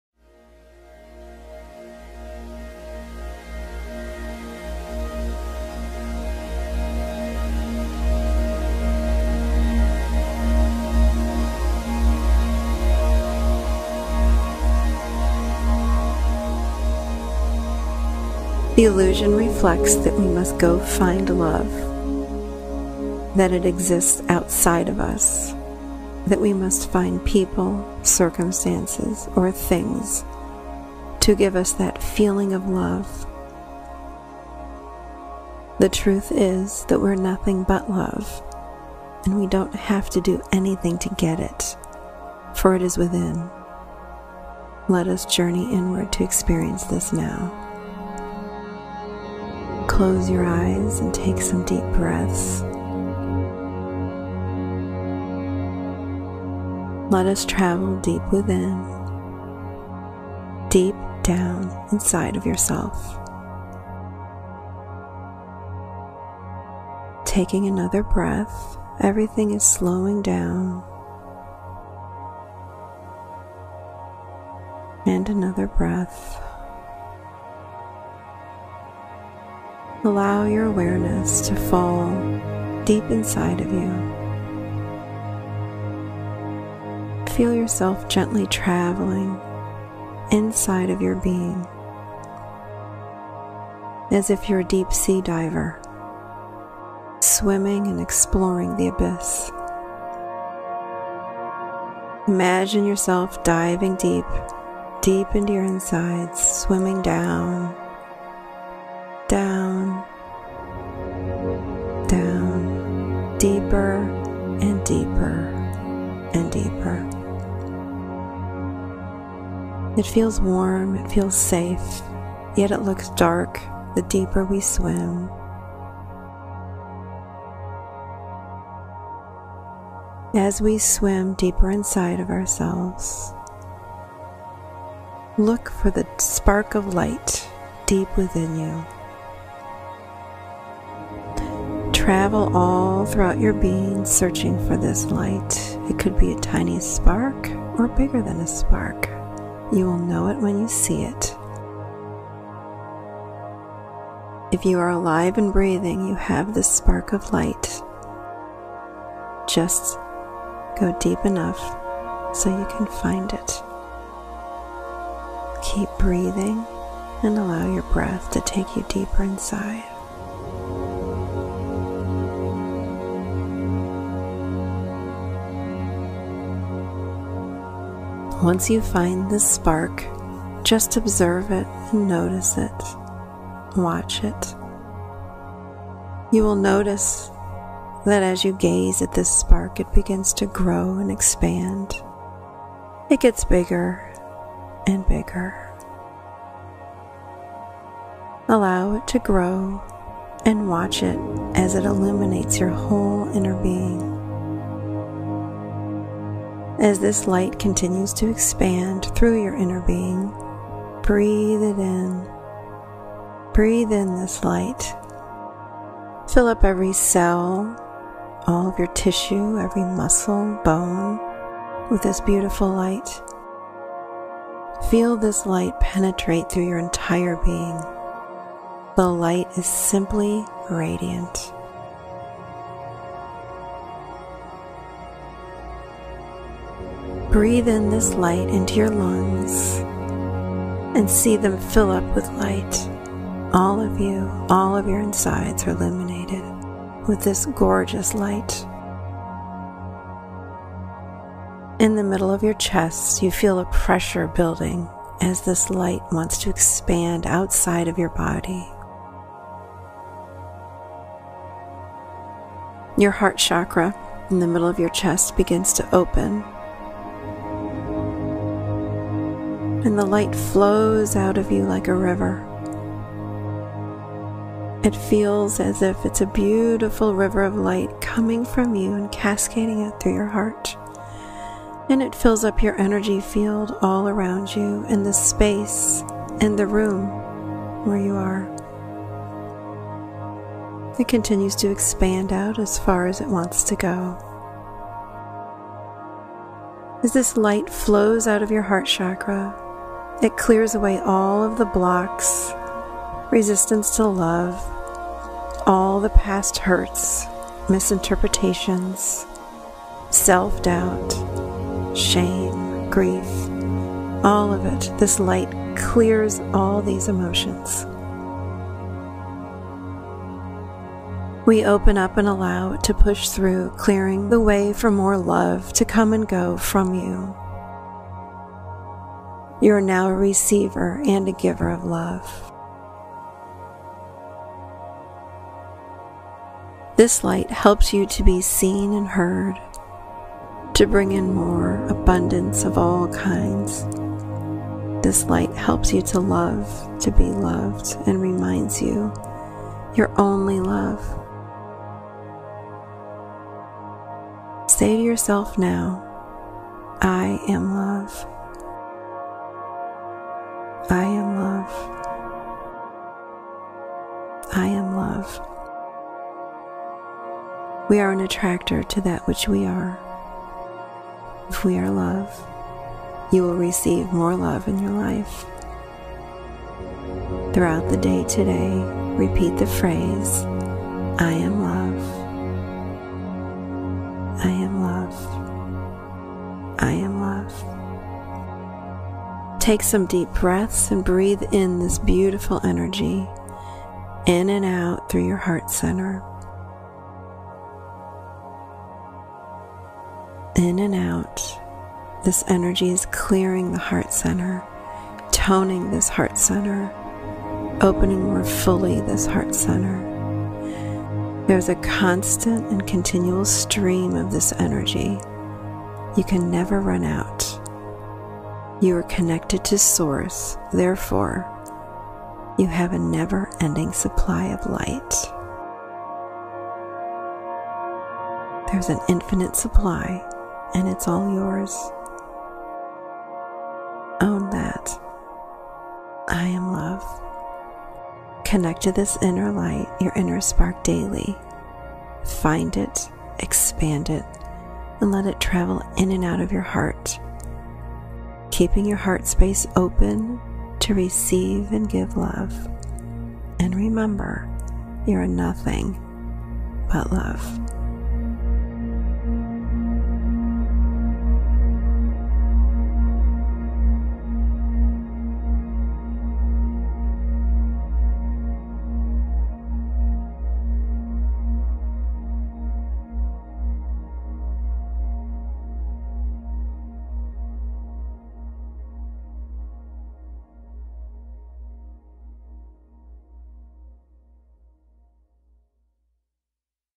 I Am Love — A Meditation with Michelle Chalfant
The illusion reflects that we must go find love, that it exists outside of us. The truth is that we are nothing but love, and we don't have to do anything to get it, for it is within.

Let us journey inward.